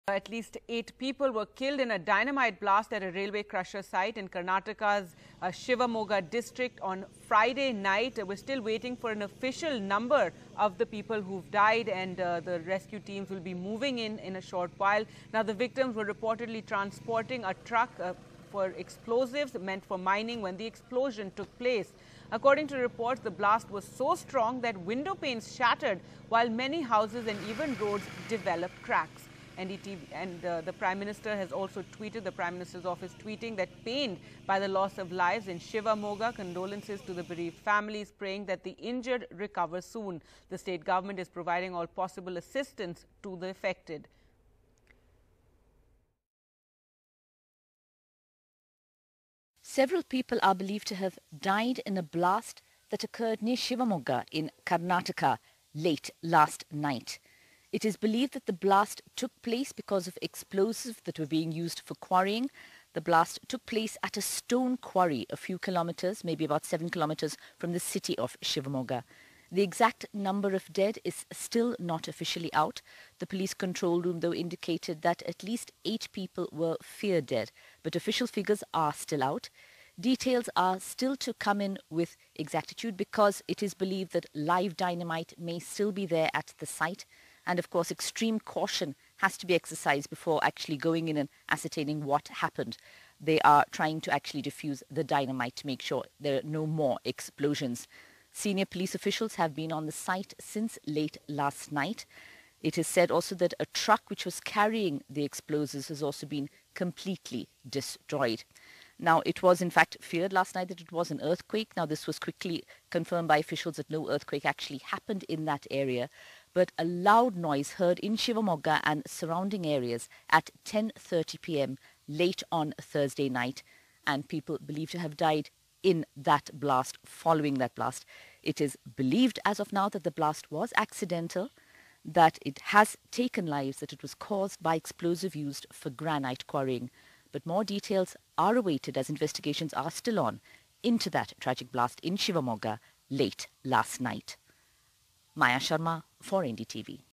8 Dead In Explosion At Quarry In Karnataka's Shivamogga, Area Sealed Off
Eight people were killed in an explosion at a stone mining quarry in Karnataka's Shivamogga district, news agency ANI reported. The possibility of subsequent explosions at the quarry at Hunasondi near village Abbalagere has not been ruled out by the police since some dynamite sticks are still live, the police said. A bomb disposal squad has been called in and the entire area has been sealed.